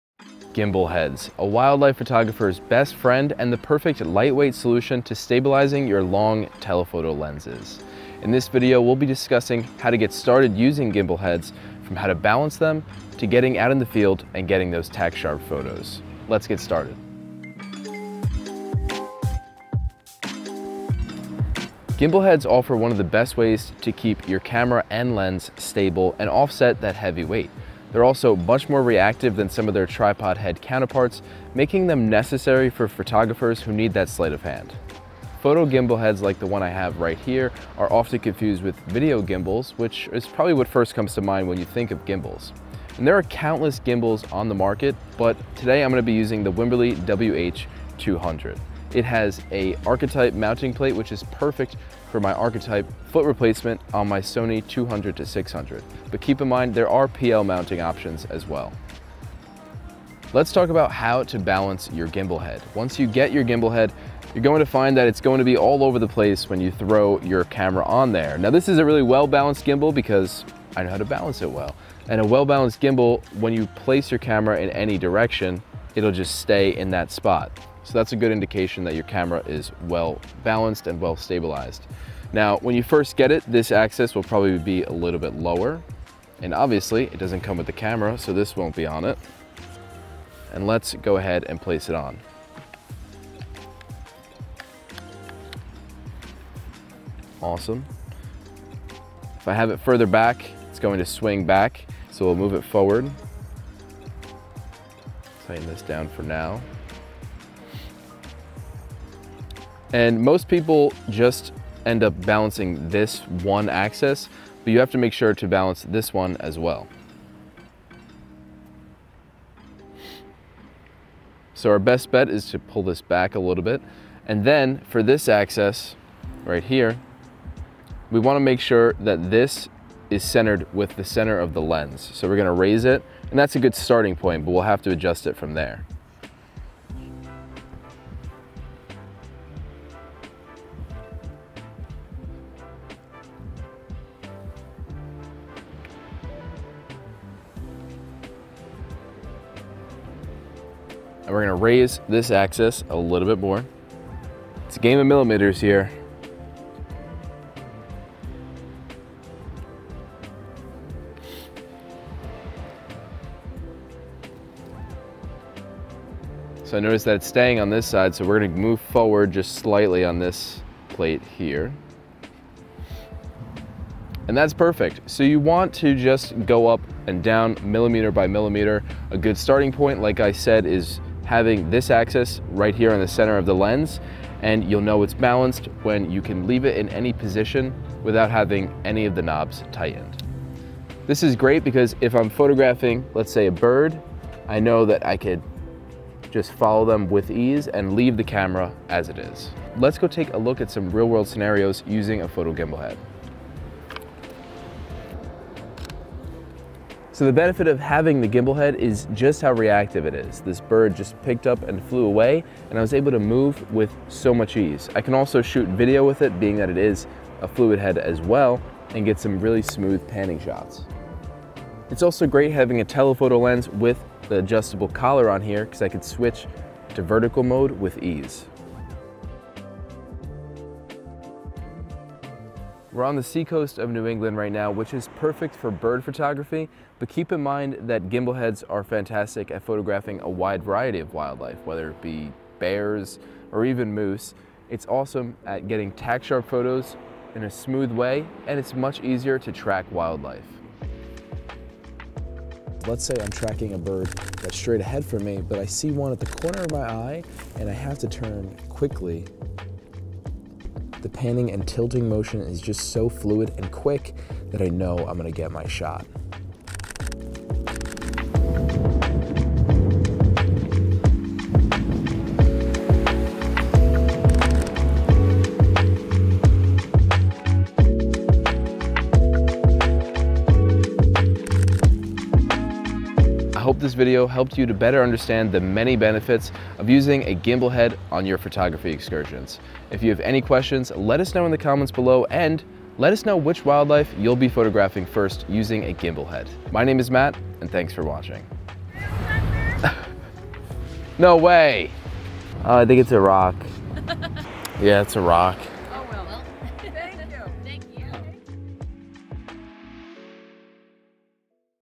Gimbal Heads: A Wildlife Photography MUST-HAVE (And How to Use It)
Want tack-sharp wildlife photos, or an easier way to hold your telephoto lens? Whether you're photographing birds or bears, gimbal heads are a great accessory to level up your photography game. In this tutorial, Matt shows you how to balance a gimbal head, as well as demonstrating its use in the field.

Gear mentioned
- Wimberley WH-200 Gimbal Tripod Head II with Quick Release Base
- Sony FE 200-600mm f/5.6-6.3 G OSS Lens

More wildlife equipment guides
- Kristi Odom's Wildlife Gear List
- Colby Brown's Wildlife & Landscape Gear

0:00 Why Gimbal Heads?
1:00 How to Balance a Gimbal Head
3:19 Real-World Scenario
4:43 Final Thoughts

Head to our B&H Event Space YouTube channel for more seminars!